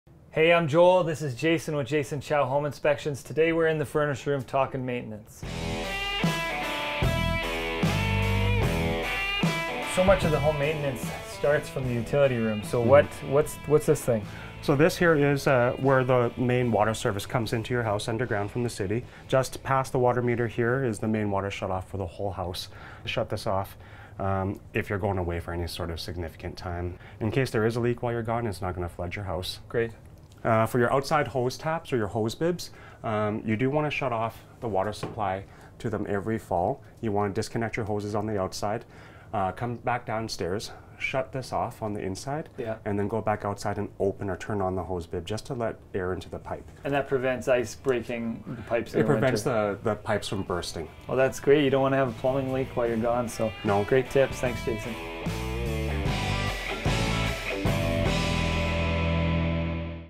Home Maintenance: Water Shutoff
Joel Anderson: Hey, I'm Joel this is Jason with Jason Chow Home Inspections. Today we're in the furnace room talking maintenance.

Joel Anderson: So much of the home maintenance starts from the utility room so what, what, what's this thing?

Jason Chow: So this here is a where the main water service comes into your house underground from the city. Just passed the water meter here is the main water shut off for the whole house. You shut this off if you're going away for any sort of significant amount of time. Incase there is a leak while you're gone it's not going to flood your house.

Joel Anderson: Great!

Jason Chow: For your outside hose taps or your hose bibs, you do want to shut off the water supply to them ever fall. You want to disconnect your hoses on the outside, come back downstairs, shut this off on the inside, and then go back outside and open or turn on the hose bib just to let air into the pipe.

Joel Anderson: And that prevents ice breaking pipes...?

Jason Chow: It prevents the pipes from busting.

Joel Anderson: Well thats great, you don't want to have a plumbing leak while you're gone so...

Jason Chow: No!

Joel Anderson: Great tips, thanks Jason!

Joel Anderson - Re/Max First
JoelAnderson.ca

Jason Chow - Home Inspector
Jchi.ca